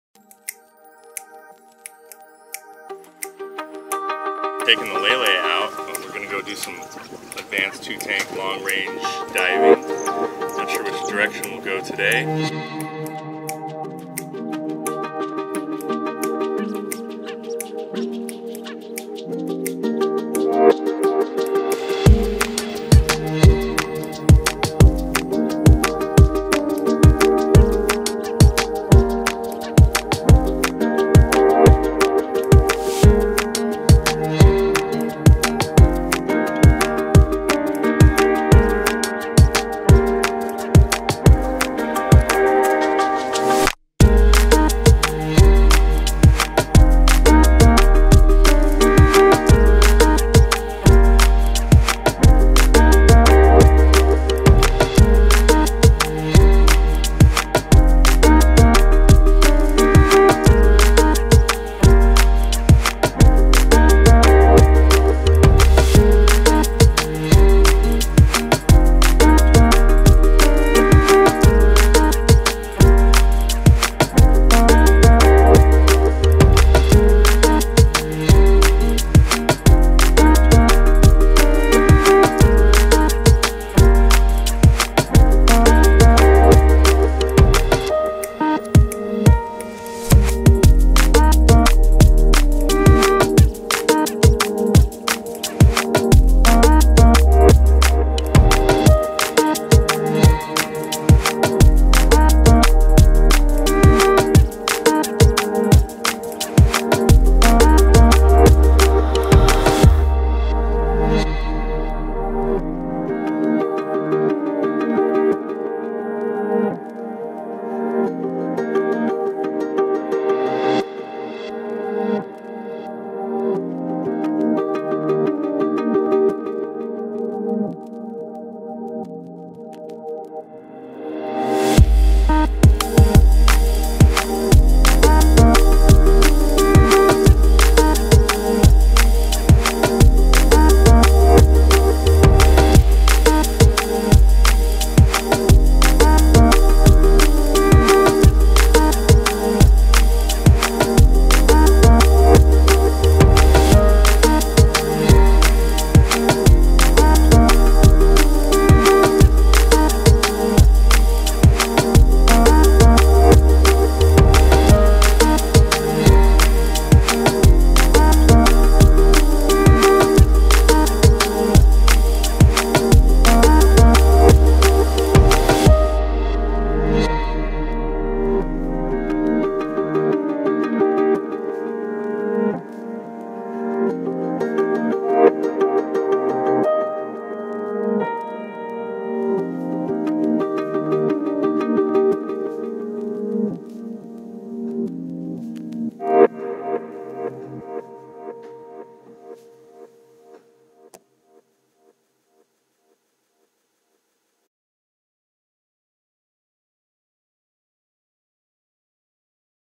Hawaii's Best Diving - Scuba Diving Kona | Premium 2 Tank Long Range
To Book This Trip

Hawaii's Best Scuba Diving is on The Big Island of Hawaii in beautiful Kona. Today we explore 2 sites: Nudibranch cavern and Casa Caves. We take you a bit further afield past all of the crowds to some of the more remote sites on the Big Island. These sites can require a bit more advanced diving. Join Kona Honu Divers for Hawaii's bes diving on one of our Premium Advanced 2-Tank Long Range Dives.